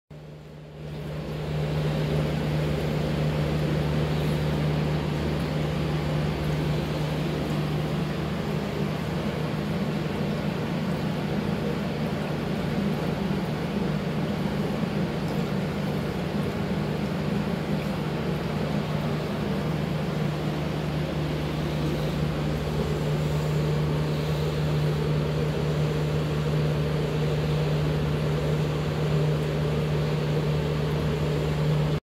80 gallon tub
80 gallon hard plastic tub
white gravel
landscape rock
eheim 350 canister filter
Livestock: bandit earth eater cichlids, albino weather loach